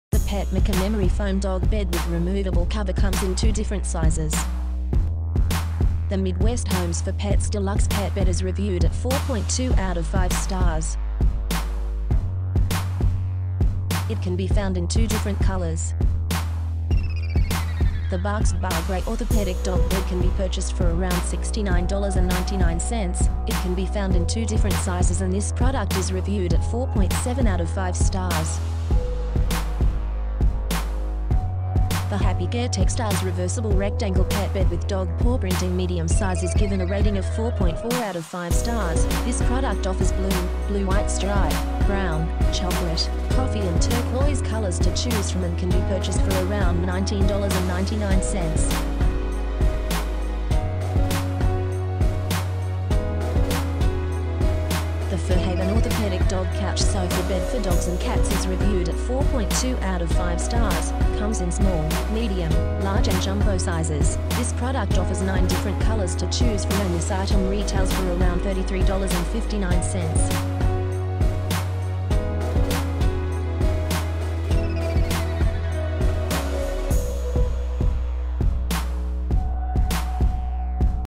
5 Top Afghan Hound Beds Reviews
Want more info on large dog beds?

Beds for large dogs are an item many people are in the market for. Some of the top large dog beds to choose from are talked about by us in this video.